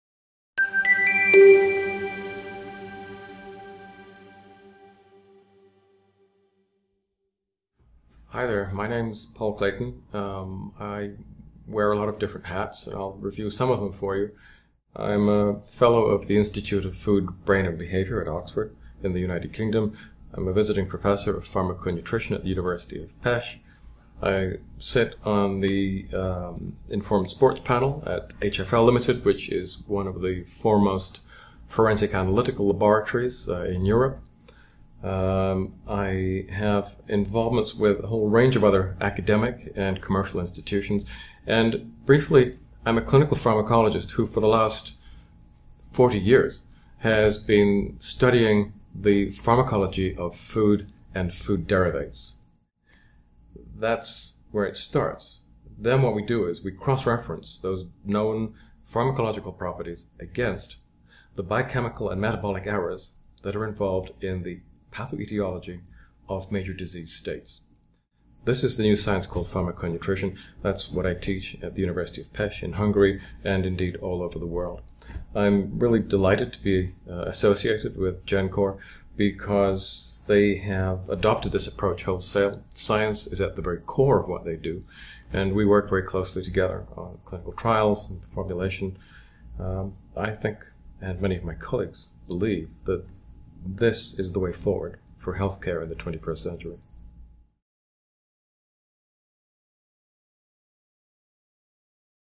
About Dr. Paul Clayton
Paul Clayton, Ph.D., Chief Scientific Advisor for Gencor. Dr. Clayton is the UK's leading expert in the fast-developing science of pharmaco-nutrition, which he teaches at the Royal College of General Practitioners. He has recently been appointed Research Director of Medical Nutrition Matters and is a Fellow of the Royal Society of Medicine.